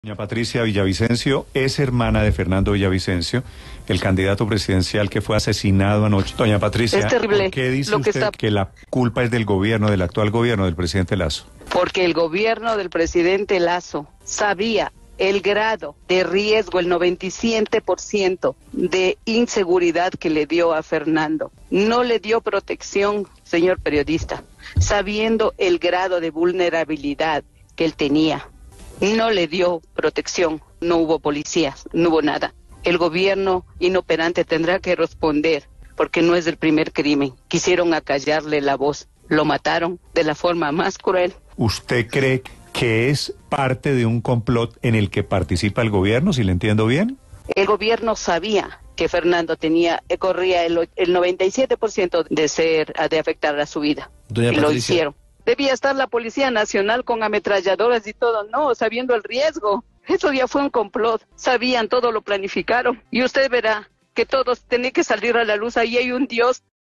El asesinato de Fernando Villavicencio fue un complot. Declaraciones de su hermana.
URGENTE

Lorena Villavicencio, hermana de Fernando Villavicencio, señala que el asesinato de su hermano, candidato presidencial 2023 en Ecuador, fue un complot.